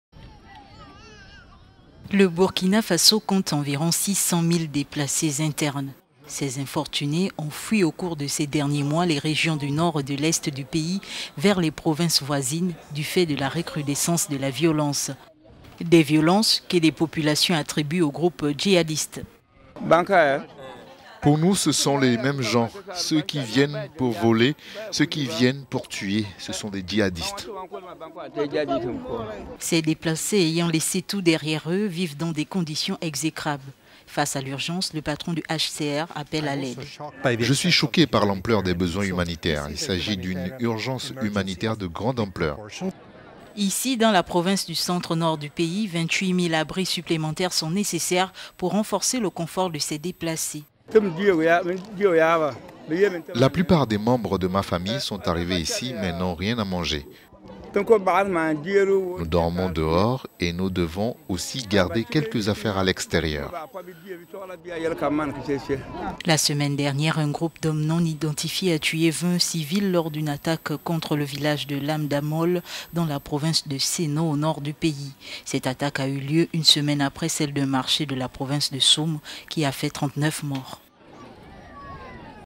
Burkina Faso : le calvaire des déplacés
Le Burkina Faso compte environ 600 000 déplacés internes suite à la montée de la violence dans le pays.*

Ces infortunés ont fui au cours de ces derniers mois les régions du nord et de l'est du pays vers les provinces voisines du fait de la recrudescence de la violence. Des violences que des populations attribuent aux groupes djihadistes.

"Pour nous, ce sont les mêmes gens, ceux qui viennent pour voler, ceux qui viennent pour tuer. Ce sont les djihadistes", affirme amidou Billarga, chef de vil…